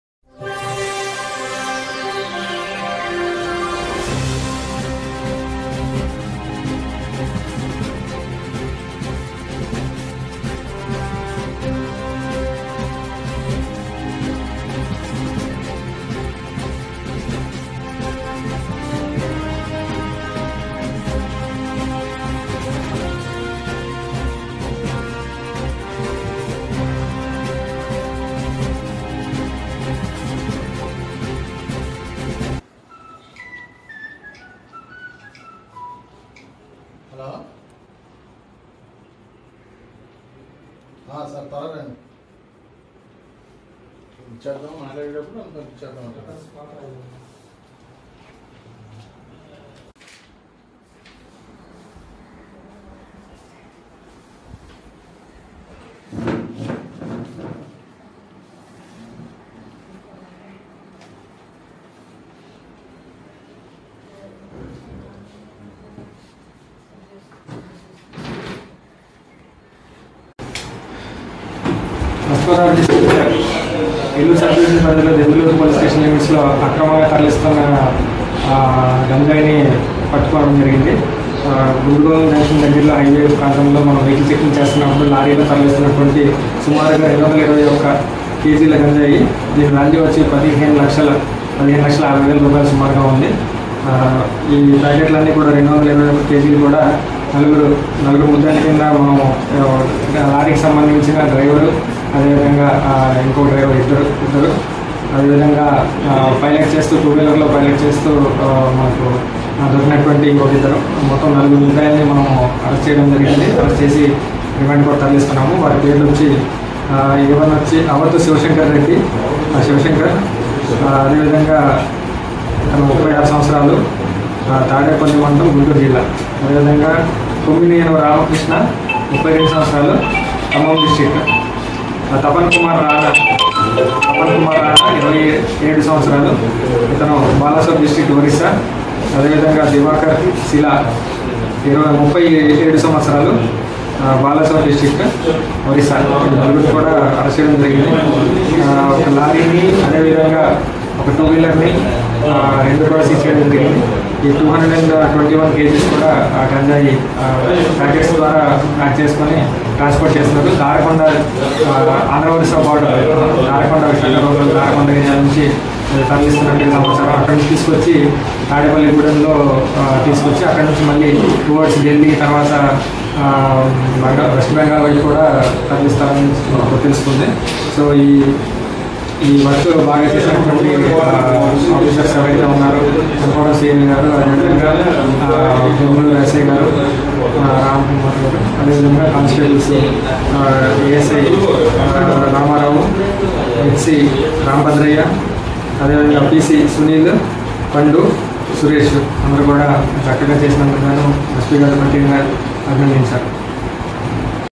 దెందులూరు పోలీస్టేషన్ పరిధిలో 15లక్షలు విలువైన 221KGల గంజాయిని తరలిస్తుండగా పట్టుకున్న పోలీసులు
ప.గోజిల్లా

దెందులూరు పోలీస్టేషన్  పరిధి
గుండుగోలను జాతీయరహదారిలో   లారీలలో అక్రమంగా తరలిస్తున్న సుమారు 15లక్షల యాభైవేలు విలువైన  221 కేజీల గంజాయిని పట్టుకున్న దెందులూరు పోలీసులు

ముందస్తు సమాచారంతో జిల్లా SP, నవదీప్ సింగ్ గ్రేవాల్ IPS,  గారి ఆదేశాలతో ఏలూరు DSO, ఓ దిలీప్ కిరణ్ పర్యవేక్షణలో భీమడోలు ఇంచార్జ్ CI, డేగక.భగవాన్ ప్రసాద్ ,దెందుకూరు SI, రామ్ కుమార్ పోలీసు సిబబ్బంది
దెందులూరు సమీపంలోని  జాతీయరహాదారిలో నిఘా ఉంచి గంజాయి అక్రమరవాణా ను అడ్డుకున్నారు..

నలుగురు  నిందితులను అరెస్ట్ చేశి  ఒక లారీ, ఒక బైక్221KGల గంజాయిని  స్వాధీనం చేసుకొన్న పోలీసులు

విశాఖ జిల్లా  దారకొండ ఏజన్సీ ఏరియానుండీ  భద్రాచలం, చింతలపూడి, వయా ఏలూరు మీదుగా తాడేపల్లిగూడెం తరలిస్తుండగా  గుండుగోలను సమీపంలో  పోలీసులకు చిక్కిన గంజాయి ముఠా...

జిల్లా SP, నవదీప్ సింగ్ గ్రేవాల్ IPS, ఆదేశాలతో ఏలూరు DSP, ఓ.దిలీప్ కిరణ్ పర్యవేక్షణలో గంజాయి స్మగ్లర్ల మూలాల కోసం మరింత లోతుగా విచారణ చేస్తున్న పోలీసు సిబ్బంది.